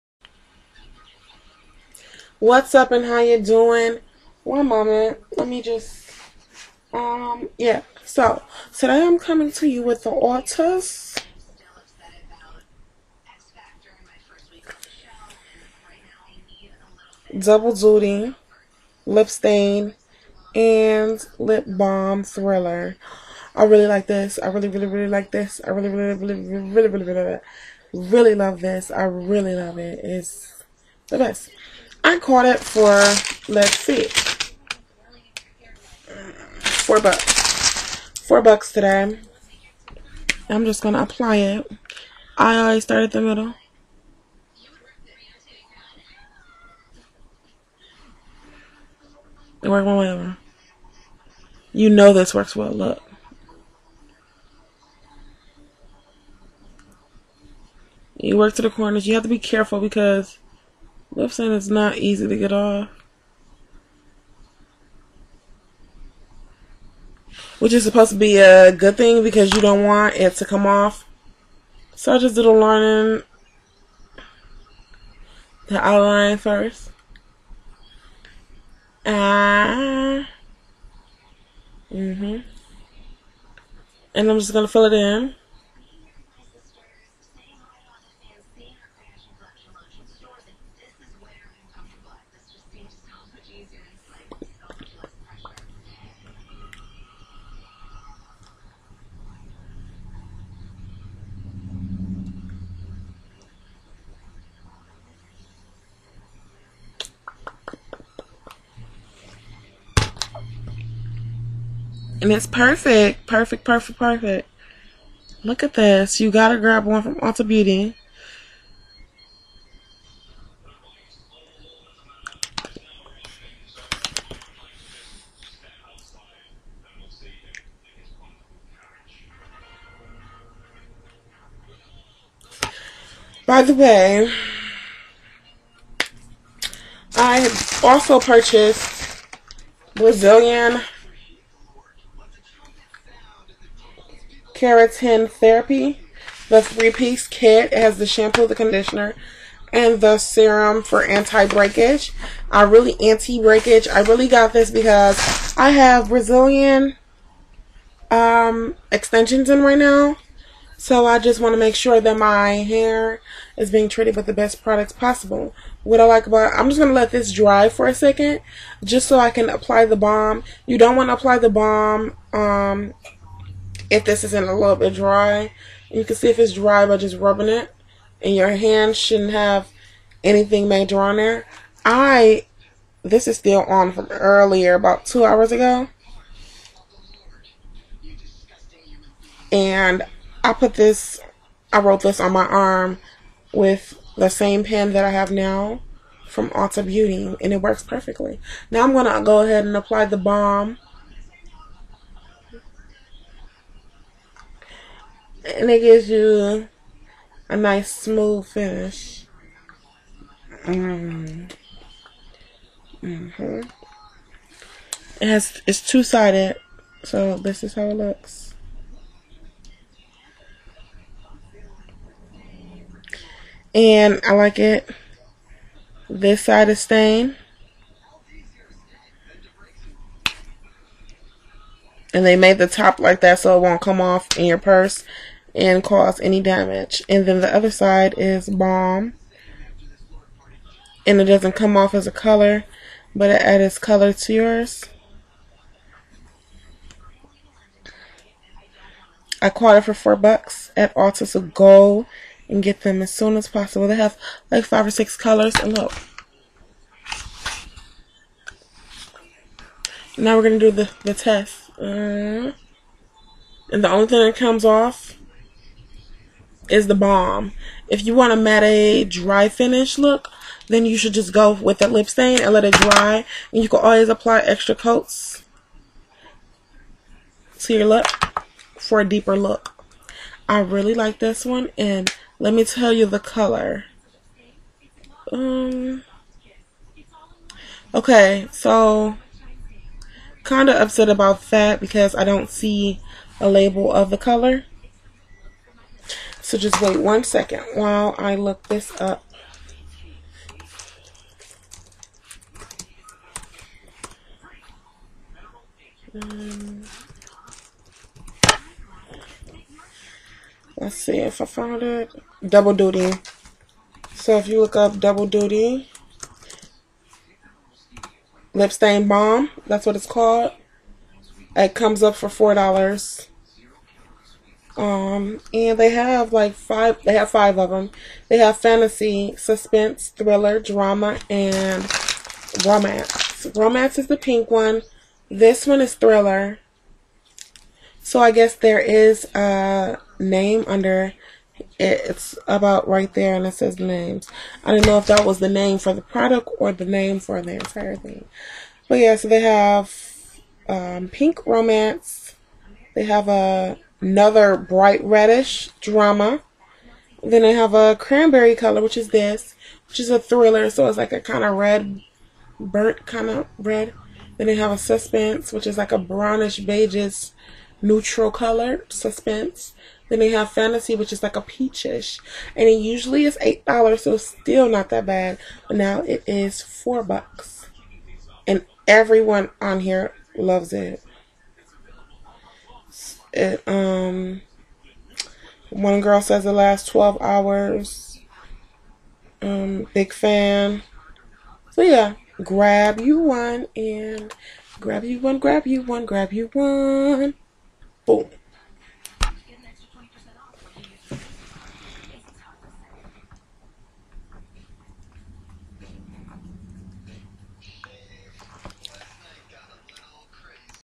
Ulta Lipstain Balm- Review
BEST LIP-STAIN!
Double Duty Lipstain & Balm by ULTA is a lip stain & balm conveniently packaged in a sleek and easy to use dual-ended applicator.